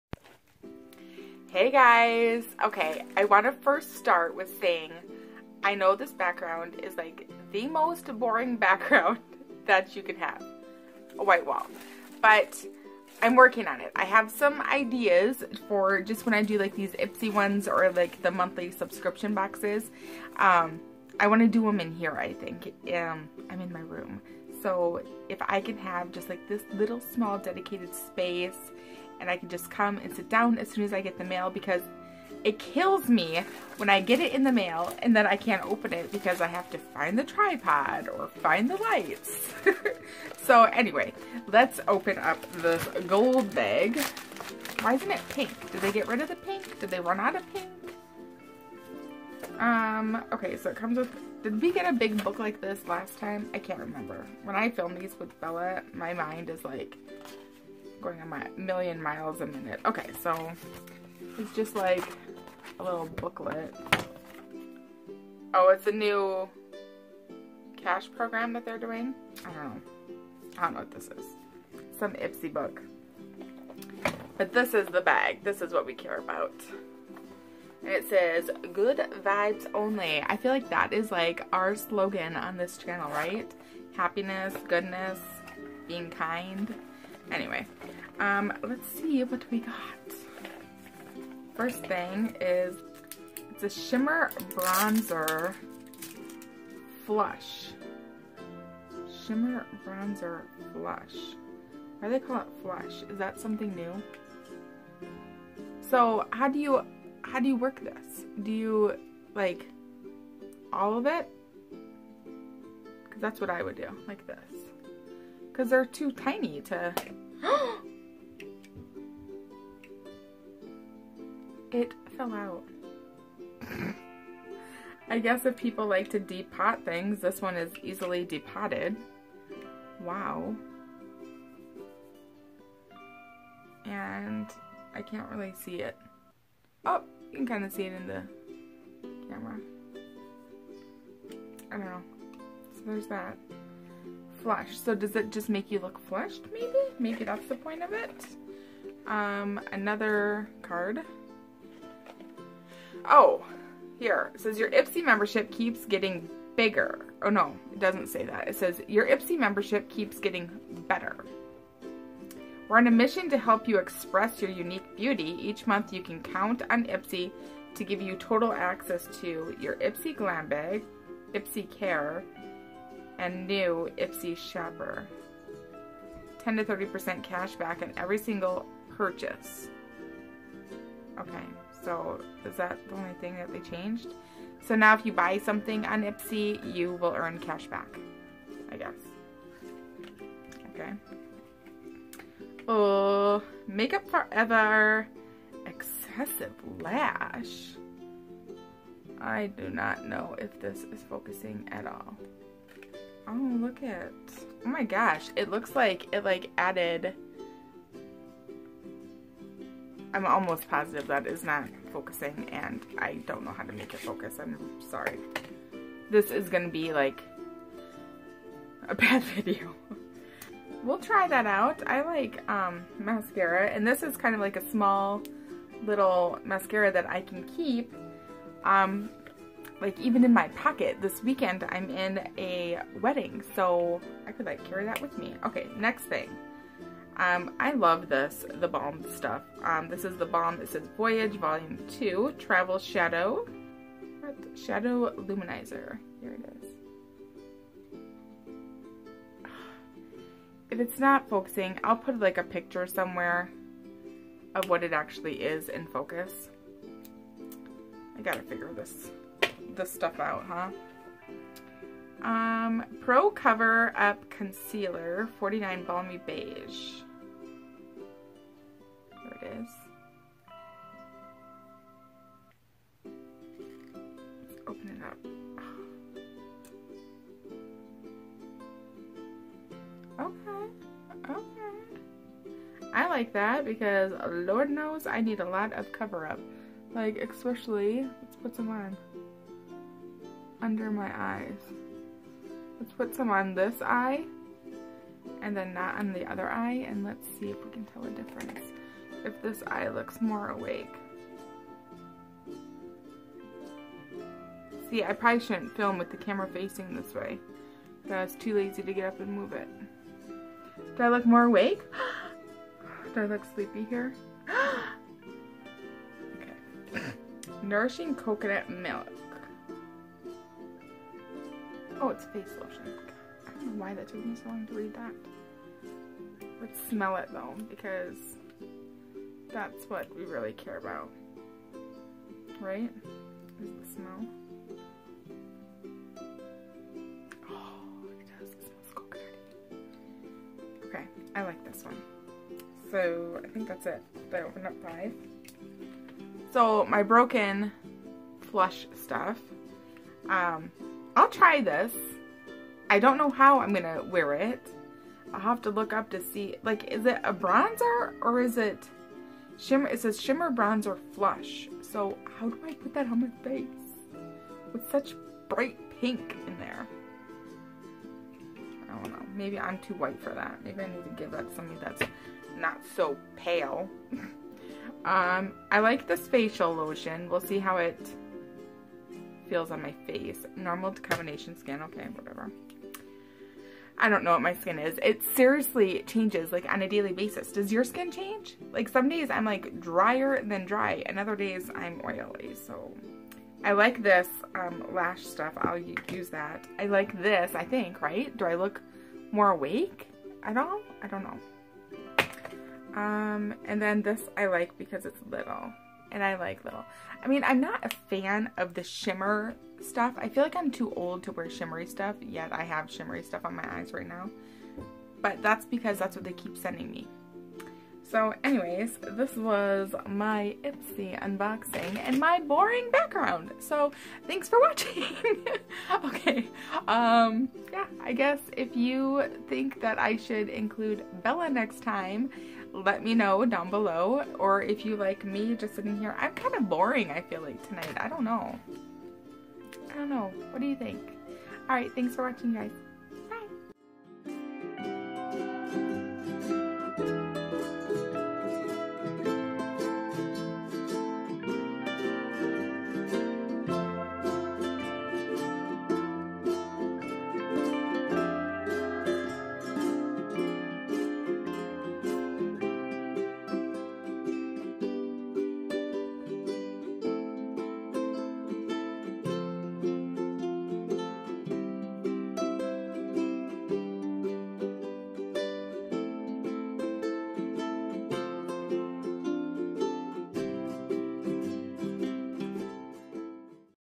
August Ipsy 2017 - Unboxing the Golden Ipsy Bag
Ipsy went gold this month

🎉💕Planning a party? Shop my shop! 💕🎉

Kiwicrate...Get $10.00 off your first month with this link!

The EYE decorations: